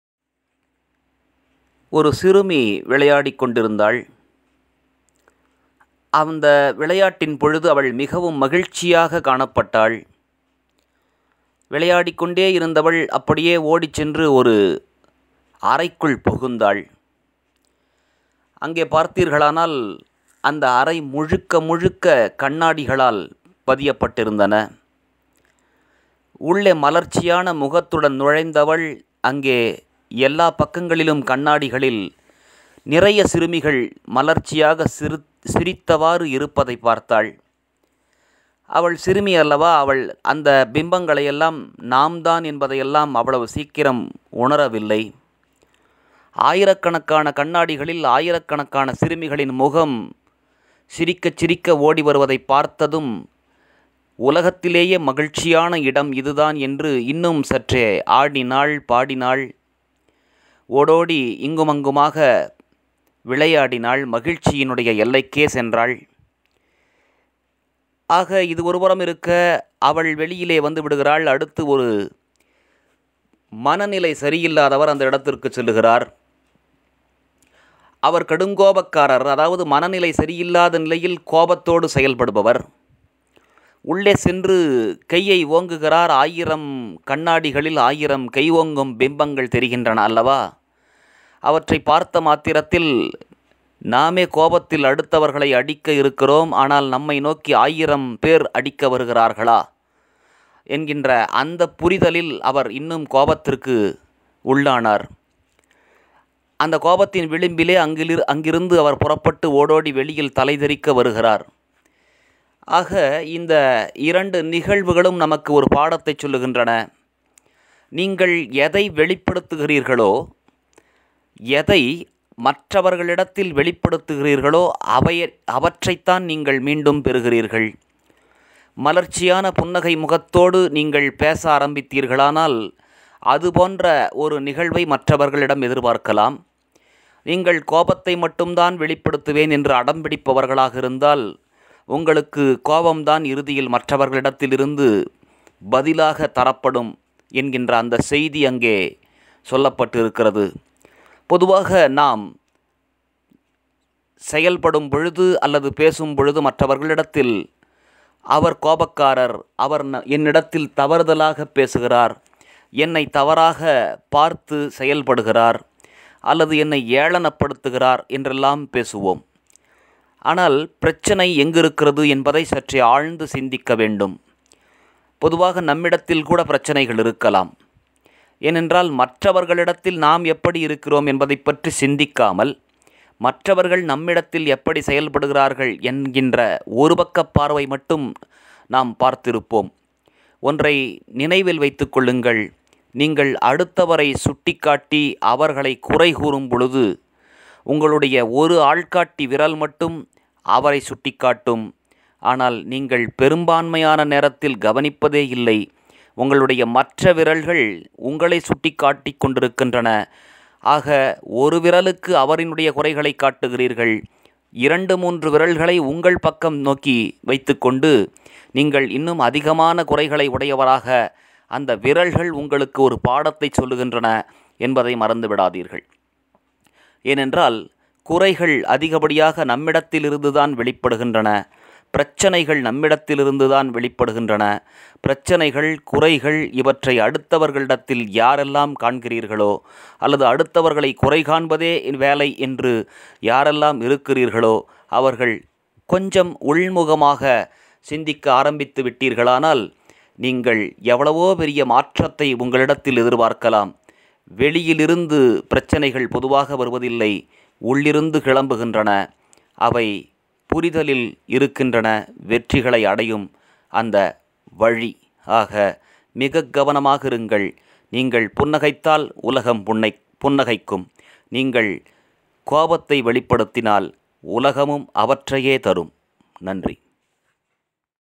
நீங்கள் புன்னகைத்தால் உலகம் புன்னகைக்கும்
இந்த உலகில் நீங்கள் எதை வெளிப்படுத்துகிறார்களோ அதையேதான் திரும்பவும் பெற்றுக்கொள்ளவும் முடியும் நீங்கள் துரோகம் அன்பு மகிழ்ச்சி சிரிப்பு கோபம் போன்ற எண்ண அலைகளை வெளிப்படுத்தினால் அந்த அலைகளை பன்மடங்காக உங்களுக்கு திருப்பி தரப்படும் அதனால் எதை வெளிப்படுத்த வேண்டும் என்பதை தீர்மானியுங்கள்